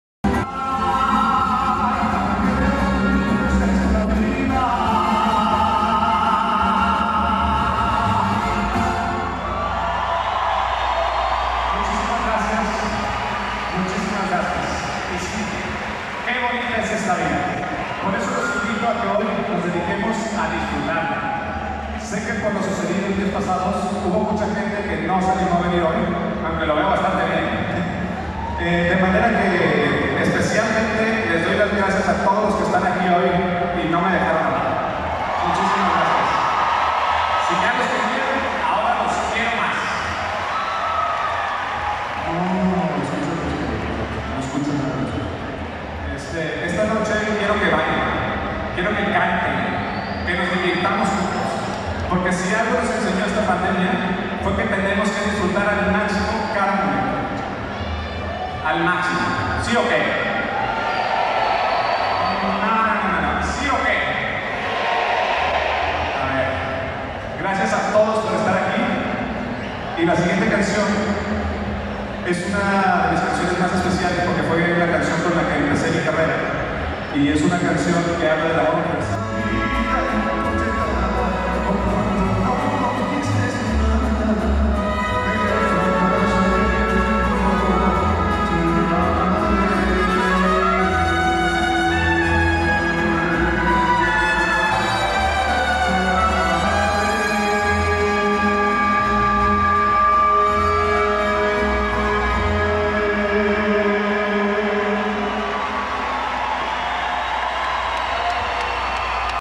Público de Guadalajara sorprende con su asistencia a Álex Fernández en Guadalajara
Alex Fernández Agradece a su público por estar presentes  y porqué no lo dejaron abajo  ante lamentables hechos que ocurrieron en días pasados en las afueras de el Palenque de Las Fiestas De Octubre en dónde cabe mencionar que tuvo una exitosa presentación cantando canciones de su abuelo @_vicentefdez .a quien recordó con un par de canciones y mencionó que gracias a él está en la música al ser su máxima inspiración.